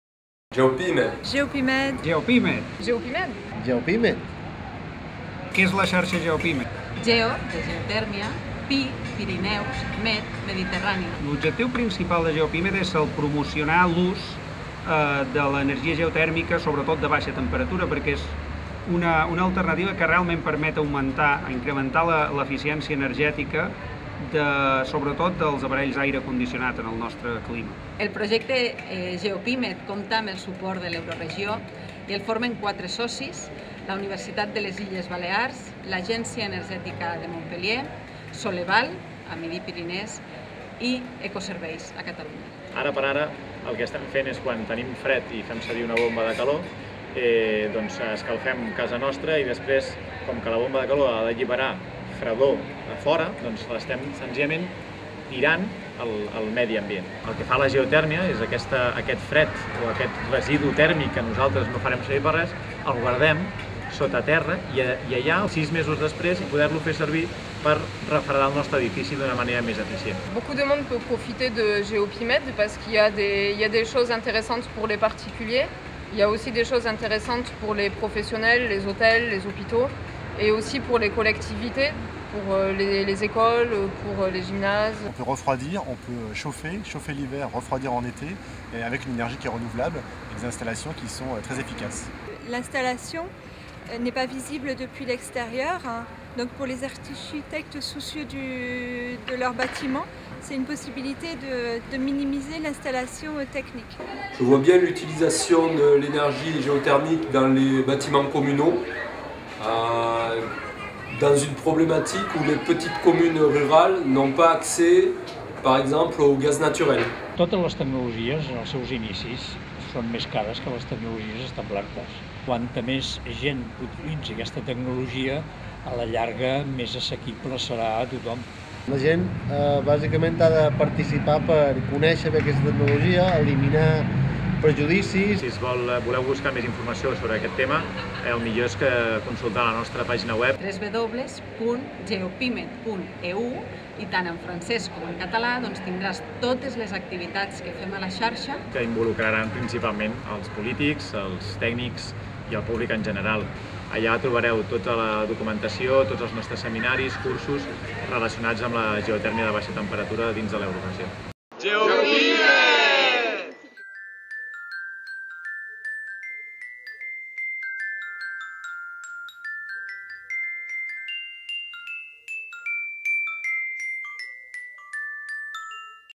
GeoPiMed vidéo (version en français)
GeoPiMed est le réseau pour la promotion de l'énergie géothermique dans les Pyrénées et la Méditerranée avec le support de l'Eurorégion.
Visitez pour en savoir plus.
En plus des vidéos et supports de documentation préparés par des experts, vous pouvez consulter la programmation des activités gratuites pour en apprendre davantage sur les nombreuses possibilités de cette énergie renouvelable.